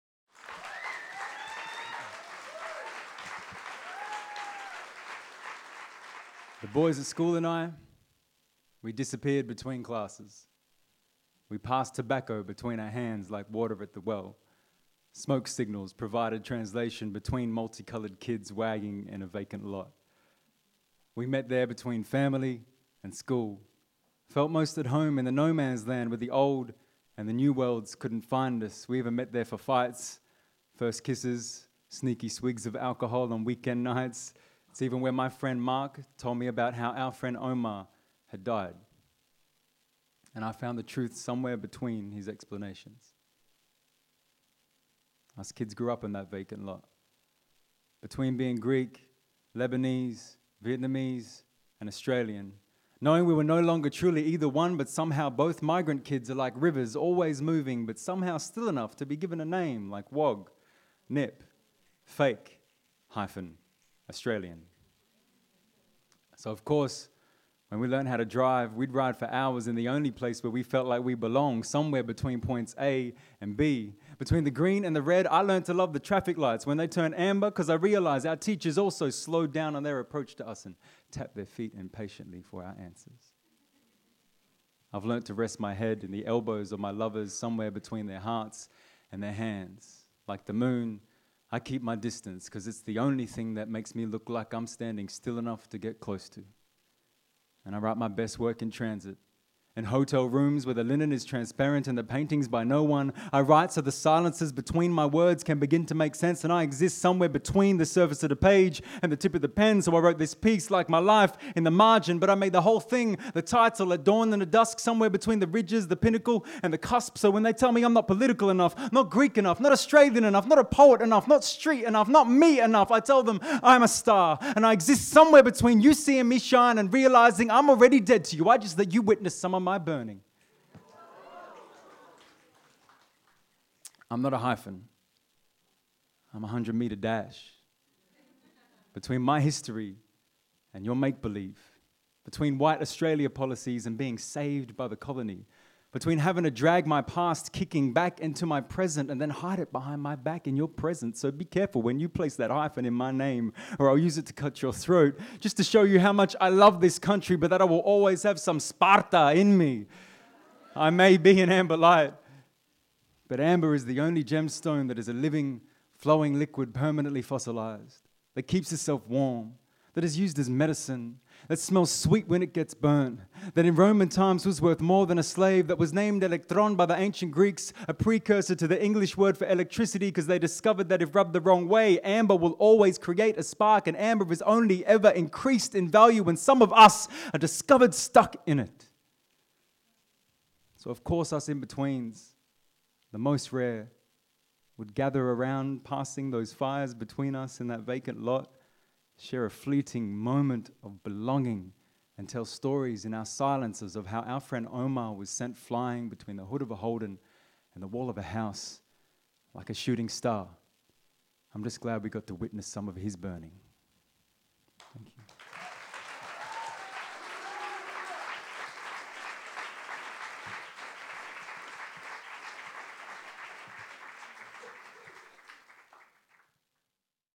Luka Lesson // Amber Lights
A M B E R    L I G H T S

The boys at school and I disappeared between classes
passed tobacco between our hands like water at the well
smoke signals provided translation
between multi-coloured kids
wagging in a vacant lot

We met there between family and school
felt most at home in the no-man's land
where the old and the new worlds couldn't find us

we even met there for fights
first kisses
swigs of alcohol on weekend nights
It's even where my friend Mark told me about how our friend Omar
had died
and I found the truth somewhere between the explanations

Us kids grew up in that vacant lot between being Greek
Lebanese
Vietnamese
and Australian

Knowing we were no longer truly either one but somehow both
Migrant kids are like rivers
always moving
but somehow still enough to be given a name
like Wog, Nip, Fake - hyphen - Australian

So of course when we learnt how to drive
we'd ride for hours in the only place where we felt like we belonged
somewhere between points A and B between the green and the red
I learnt to love the traffic lights when they turned amber because I realised our teachers also slowed down on their approach to us
and tapped their feet impatiently for our answers

I've learnt to rest my head in the elbows of my lovers
somewhere between their hearts and their hands
like the moon
because it's the only thing that makes me look
like I'm standing still enough to get close to

So now I write my best work in transit
in hotel rooms where the linen is transparent and the paintings by no-one

I write so the silences between my words can begin to make sense and I exist somewhere between the surface of the page
and the tip of the pen
so I wrote this piece - I the margins

but the whole thing
is the TITLE
at dawn and at dusk
somewhere between the ridges
the pinnacle and the cusp

So when they tell me I'm not political enough
not Greek enough
not Australian enough
not a poet enough
not street enough or not me...enough

I tell them I am a star
and I exist somewhere between you seeing me shine and realising
I'm already dead to you I just let you witness some of my burning

I am not a hyphen
I am a 100-metre dash
between my history
and your make believe

between White-Australia policies and being saved by the colony

between having to drag my past, kicking, back into my
present
and then hide it behind my back
So be careful when you place that hyphen in my name
or I'll use it to cut your throat
just to show you how much I love this country
but that I will always have some Sparta in me
I may be the amber light
but amber is the only gemstone that is a living, flowing liquid
permanently fossilised
that keeps itself warm
that is used as medicine
that smells sweet when it gets burnt
that in Roman times was worth more than a slave
that was used to decorate Mycenaean tombs
that was named 'elektron' by the Greeks a precursor to the English word for electricity because they discovered that if rubbed the wrong way
Amber will always create a spark
and amber is only ever increased in value
when some of us
are discovered stuck in it

So of course us forgotten ones
the in-betweens
the most rare
would gather around the fires in that vacant lot
and tell stories in our silences
of how our friend Omar was sent flying
between the hood of a Holden
and the wall of a house
like a shooting star

I'm just glad we got to witness some of his burning...